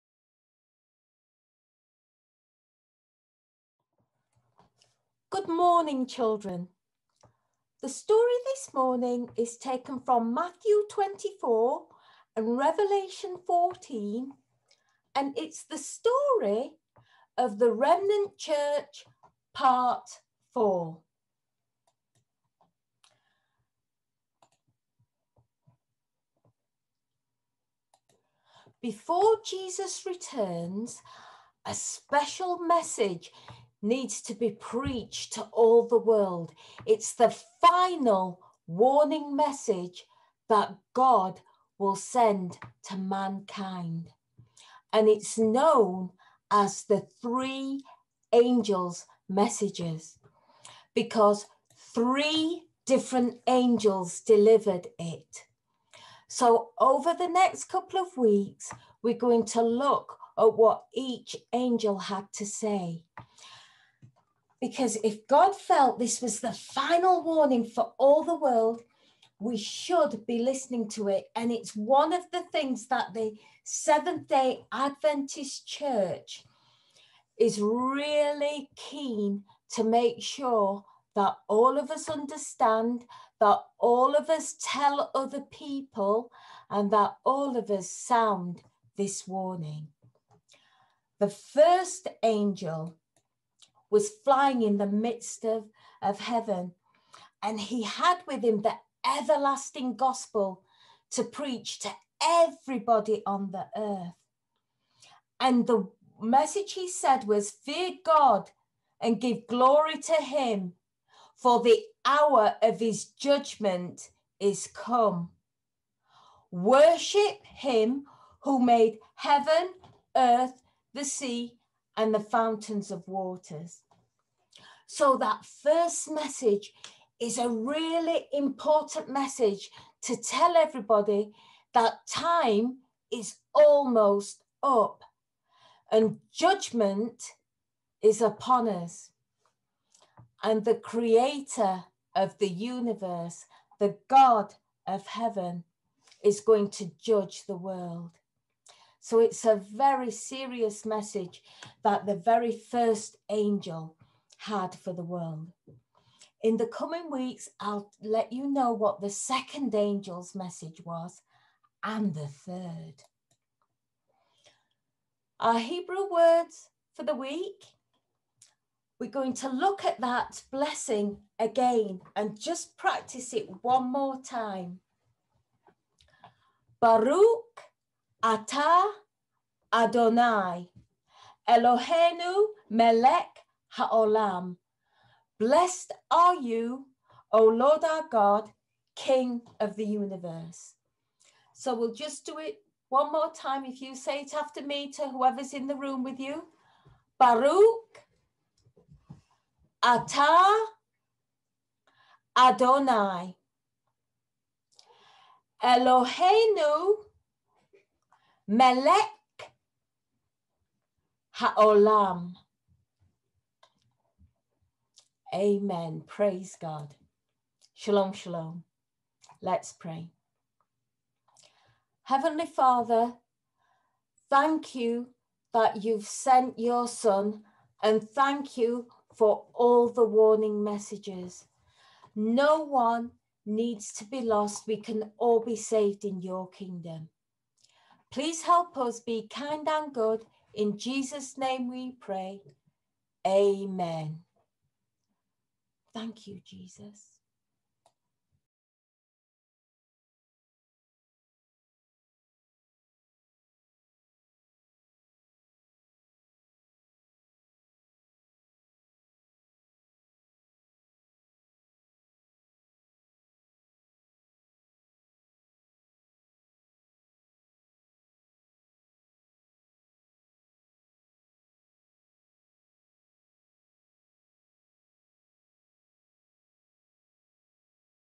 Remnant Church Part 4 | Maureen Howard | Children Story | Carter Knowle Adventist Church | 19/06/21
This message was recorded during an online Family Service at Carter Knowle Seventh-day Adventist Church.